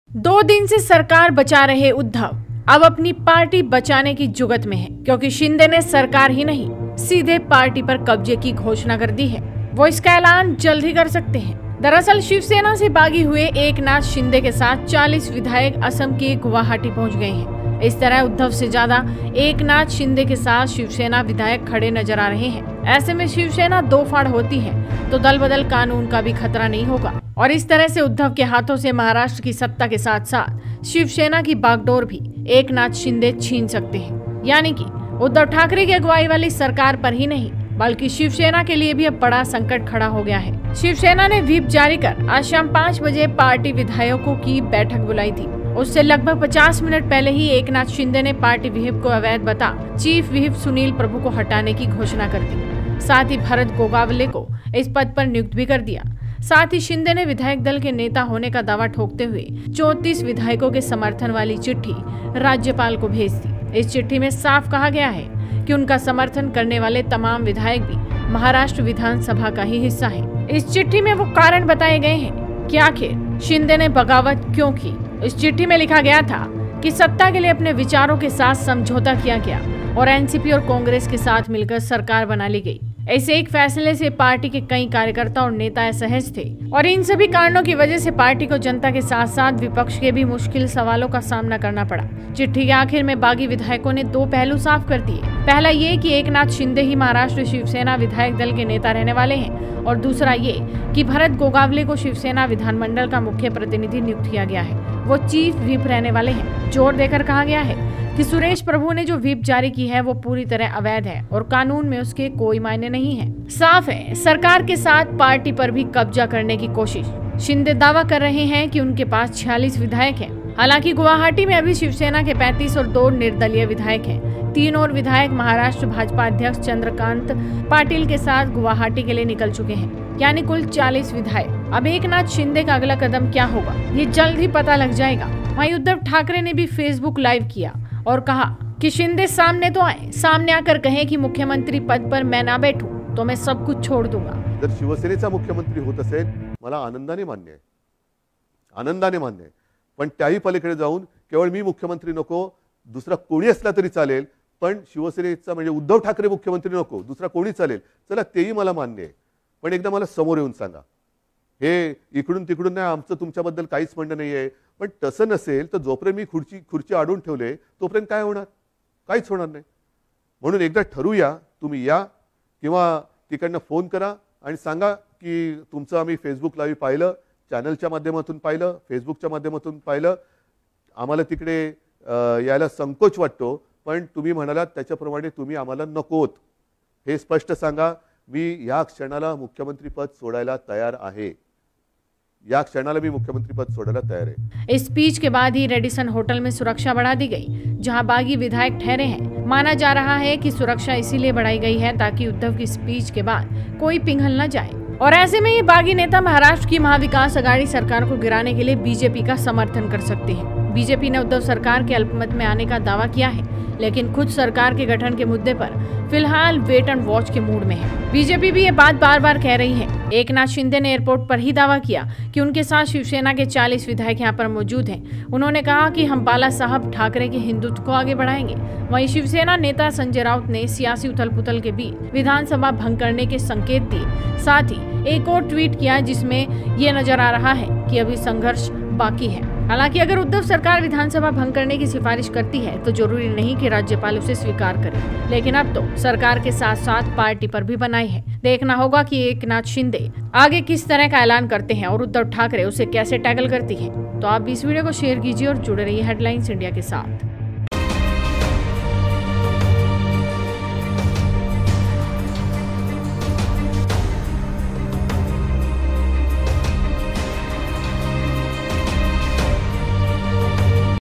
Eknath Shinde ने कर ली तैयारी, अब शिवसेना भी छिनेगी उद्धव से ! Headlines India
Eknath Shinde ने कर ली तैयारी, अब शिवसेना भी छिनेगी उद्धव से. Must Watch This Full Video